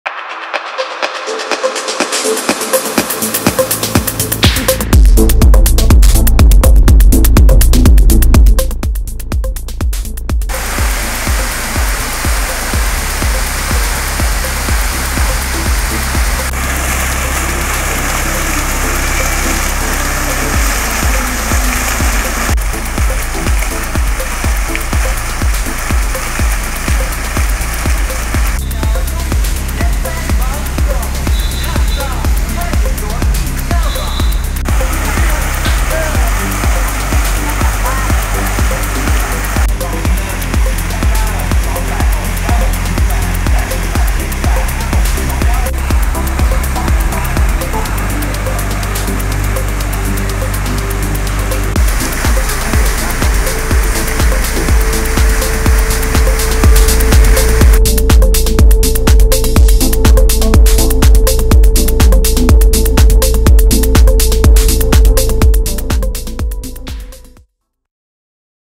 LES FONTAINES DE UD TOWN
UD TOWN est un complexe de restaurants, salons de beauté, épicerie et marché public. Dans l'allée qui circule au centre du complexe on retrouve 7 fontaines qui animent de leur musique les gens qui y déambulent.